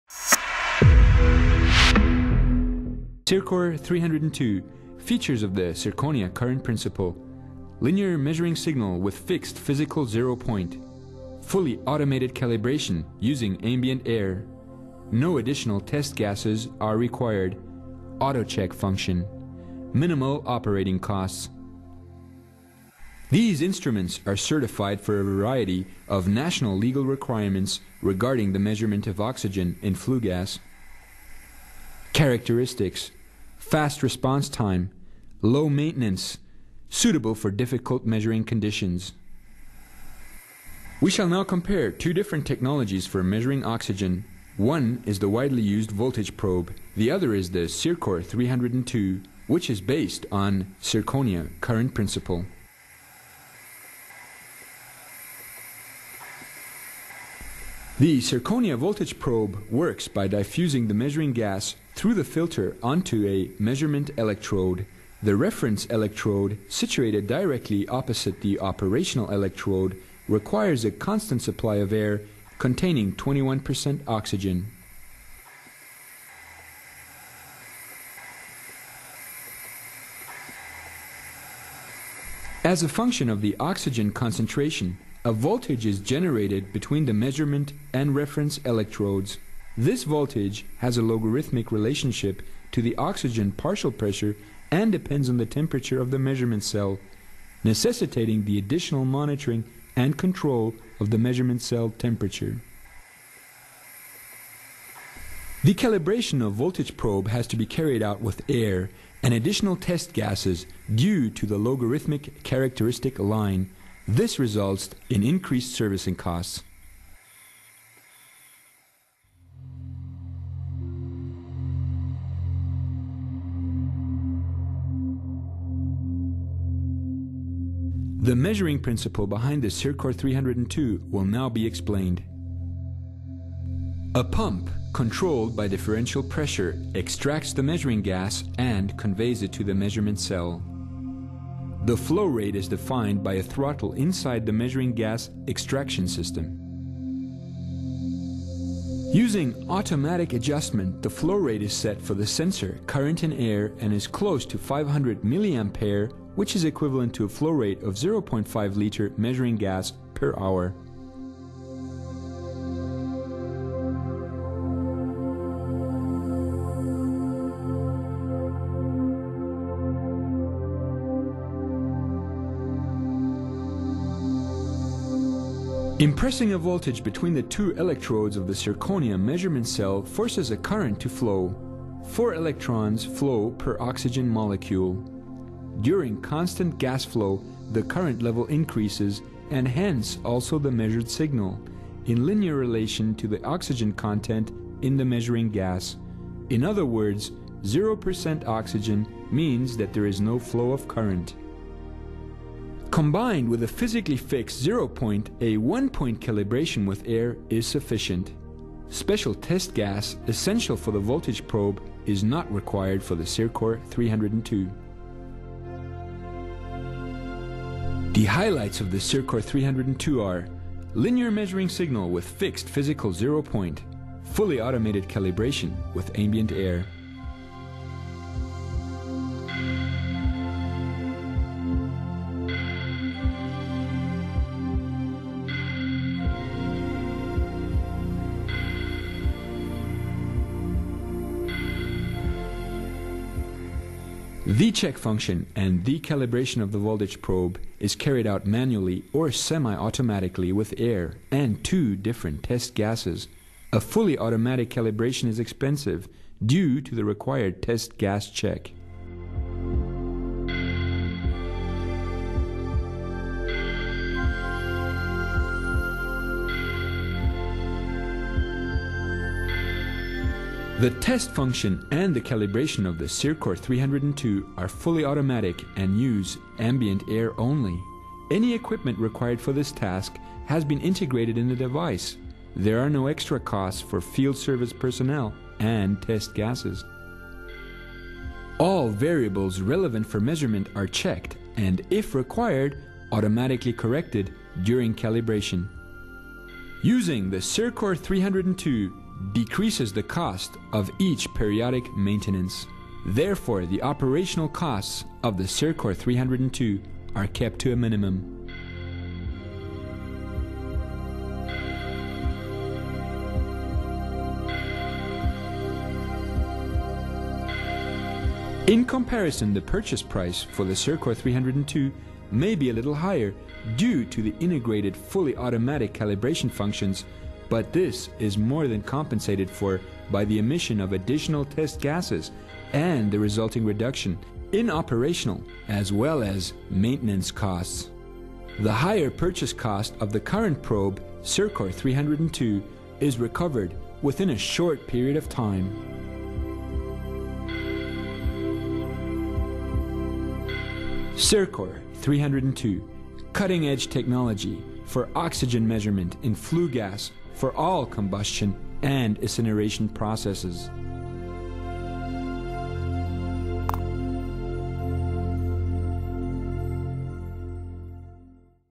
Oxygen measurement with ZIRKOR302 from SICK | SICK AG
The ZIRKOR302 in-situ analyzer provides a reliable and rapid measurement of oxygen, also at higher temperatures. Designed in line with current safety standards the analyzer does not represent a potential ignition source in the sample gas. An optional evaluation unit enables remote control of up to three analyzers. Two variants of the analyzer are available:

    ZIRKOR302-P with integrated sample gas pump
    ZIRKOR302-E with an ejector driven by compressed air

SICK AG
Erwin-Sick-Str. 1
79183 Waldkirch, Germany